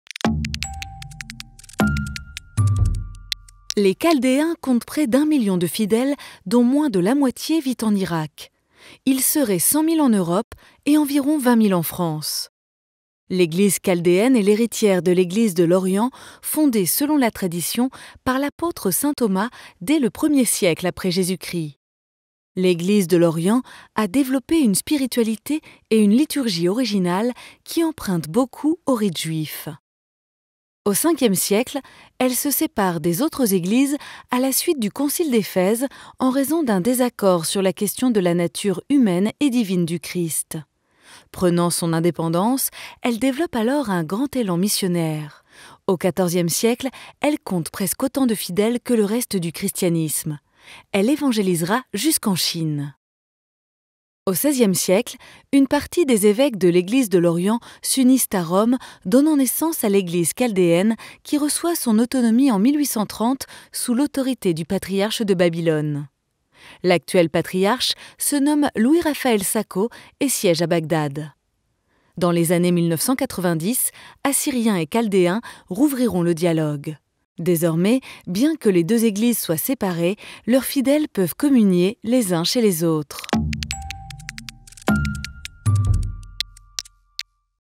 Qui sont les chaldéens ? | Le tour de la question
Lors de sa visite historique en Irak, le pape François a célébré, samedi 6 mars 2021, une messe selon le rite chaldéen à la cathédrale Saint-Joseph de Bagdad. C'est une première pour un pape. Qui sont les chaldéens ? Faisons le tour de la question.

Les chaldéens comptent près d’un million de fidèles, dont moins de la moitié vit en Irak. Ils seraient 100 000 en Europe et environ 20 000 en France.

L’Église chaldéenne est l’héritière de l’Église de l’Orient fondée, selon la Tradition, par l’apôtre saint Thomas, dès le premier siècle après Jésus-Christ.

L’Église de l’Orient a développé une spiritualité et une liturgie originale qui emprunte beaucoup aux rites juifs.

En 431, elle se sépare des autres Églises réunies au concile d’Éphèse en raison d’un désaccord sur la question de la nature humaine et divine du Christ.

Prenant son indépendance, elle développe alors un grand élan missionnaire : au XIVe siècle, elle compte presque autant de fidèles que le reste du christianisme. Elle évangélisera jusqu’en Chine !

Au XVIe siècle, une partie des évêques s’unissent à Rome, donnant naissance à l’Église chaldéenne, qui reçoit son autonomie en 1830 sous l’autorité du patriarche de Babylone. L’actuel patriarche se nomme Louis Raphaël Sako et siège à Bagdad.

Dans les années 1990, assyriens et chaldéens rouvriront le dialogue.

Désormais, bien que les deux Eglises soient séparées, leurs fidèles peuvent communier les uns chez les autres.